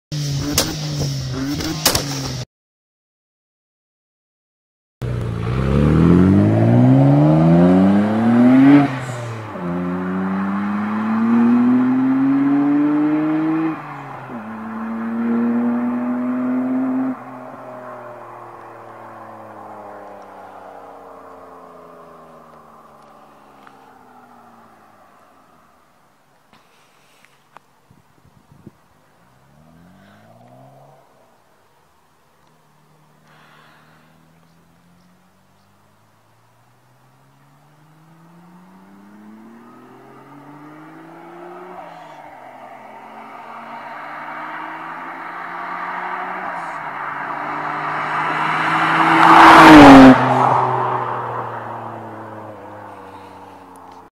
Skyline GTR Pulling away sound and drive by
Just a quick video of a pull from 0-60 and then a drive by on throttle.